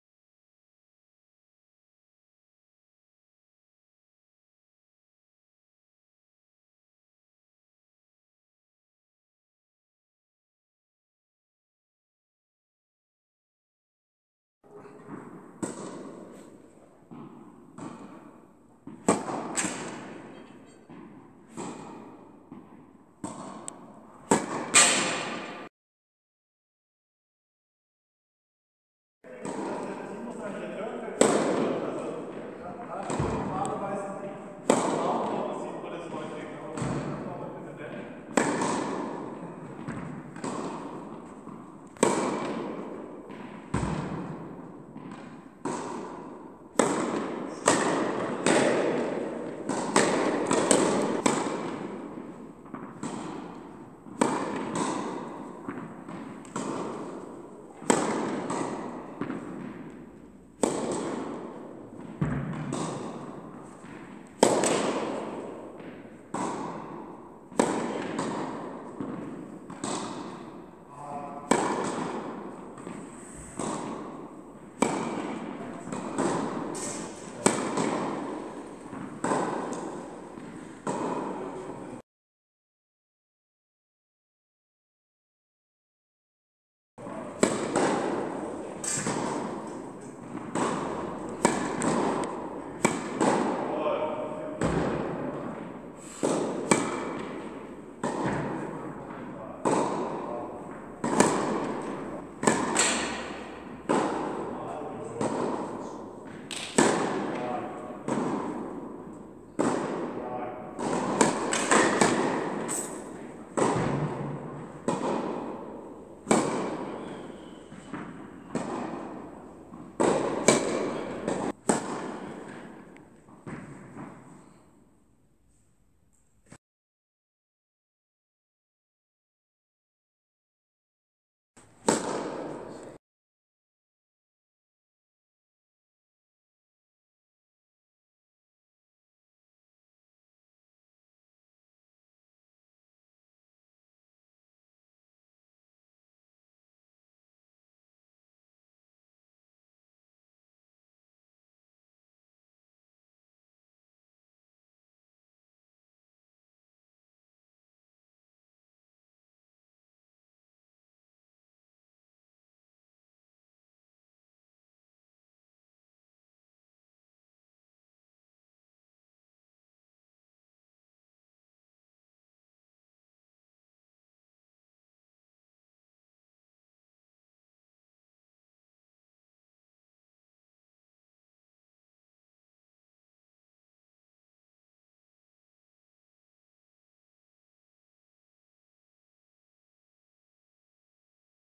Technical Training - Corrective Drill for a low Elbow in the Trophy Position
Hey guys, here is a video with a corrective drill for players, which drop their elbow too low in the trophy position. You can use your students smash to change this very quickly. So just check it out. If you want further information about the "RoPe Tennis Education" project, pleas watch our series "What we want", or send an email to
Many happy returns  ;)  Cheers guys